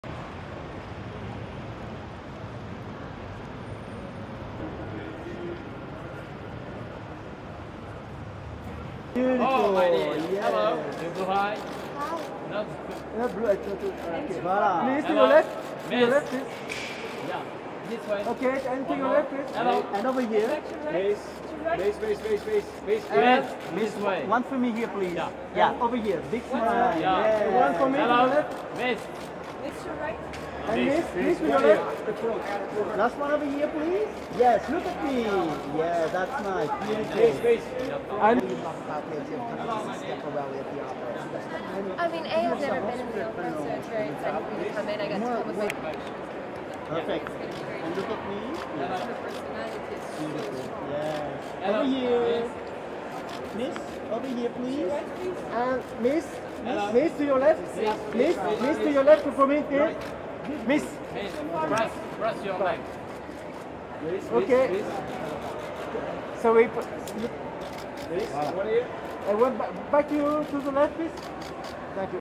Anya Taylor-Joy and Alice Eve at Schiaparelli Fashion Show in Paris
Anya Taylor-Joy and Alice Eve at Schiaparelli Spring Summer 2019 Haute Couture Photocall at the Opera Garner in Paris
Paris, France on Monday July 2, 2018